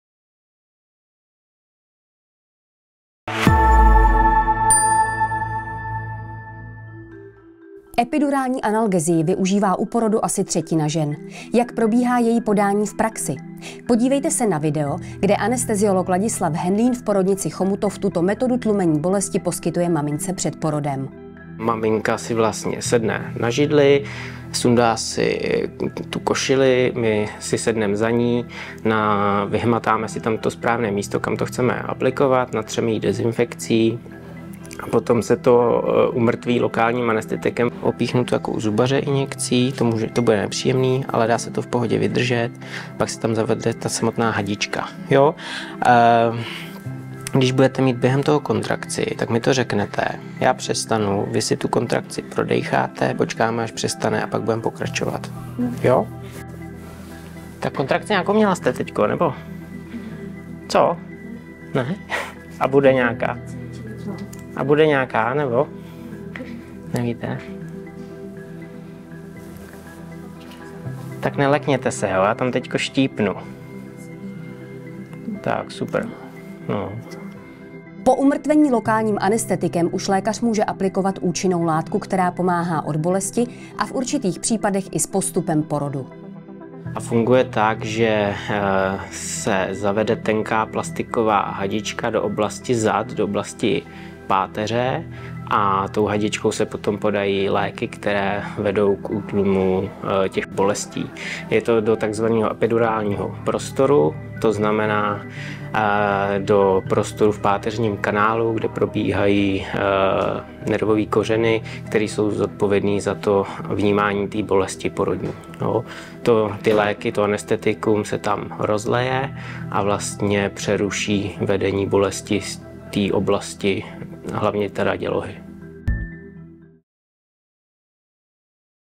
Epidurál v přímém přenosu! Takhle se aplikuje v chomutovské porodnici
Epidurální analgezii využívá u porodu asi třetina žen, jak probíhá její podání v praxi? Podívejte se na video, kde anesteziolog Ladislav Henlín v porodnici Chomutov tuto metodu tlumení bolesti poskytuje mamince před porodem.